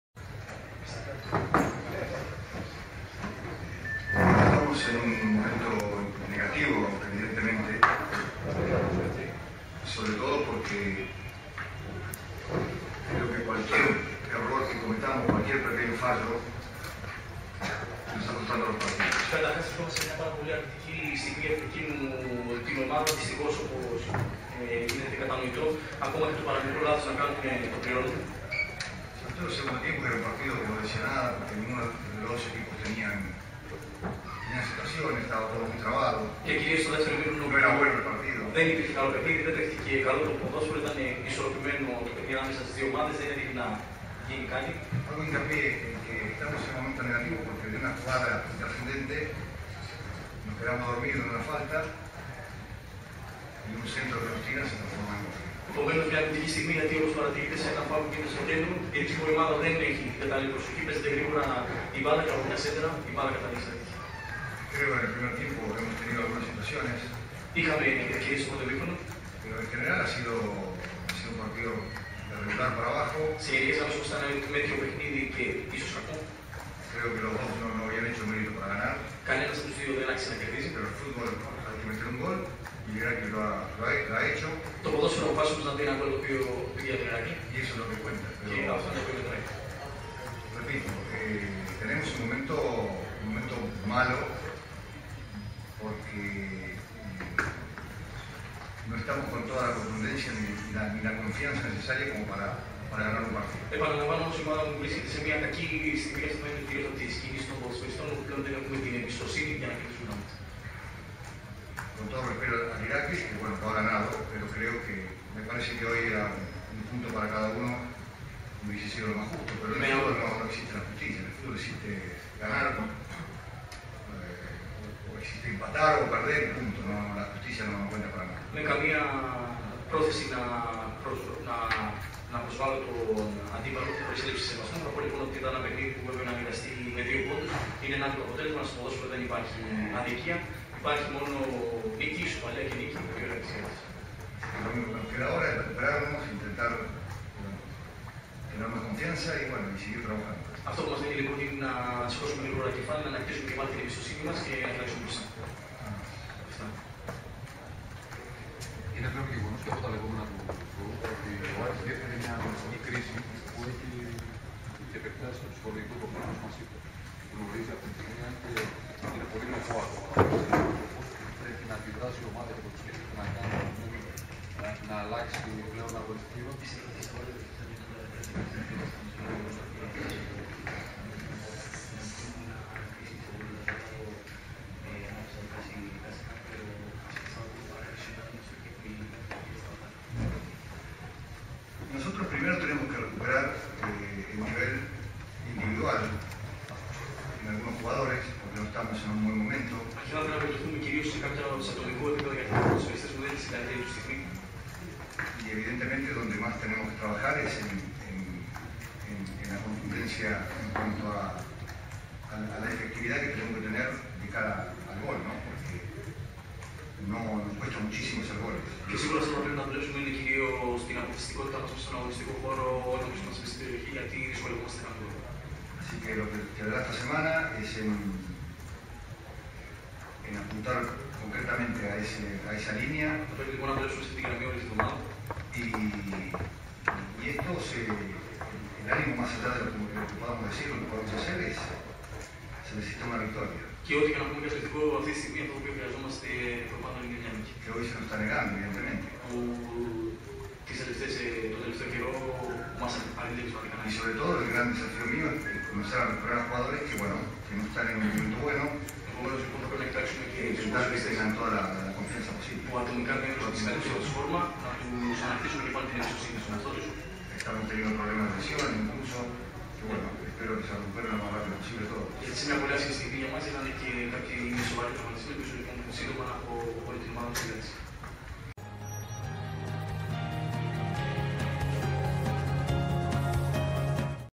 Iraklis Aris: Oi diloseis toy Hector Cuper stin Synenteyxi Typoy - sportena.com
Απόσπασμα από τις δηλώσεις του Έκτορ Κούπερ στην Συνέντευξη Τύπου, μετά τη λήξη του ντέρμπι Ηρακλής - Άρης στο Καυταντζόγλειο στάδιο για τη 10η αγωνιστική της Super League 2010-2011. Όλα τα βίντεο του αγώνα εδώ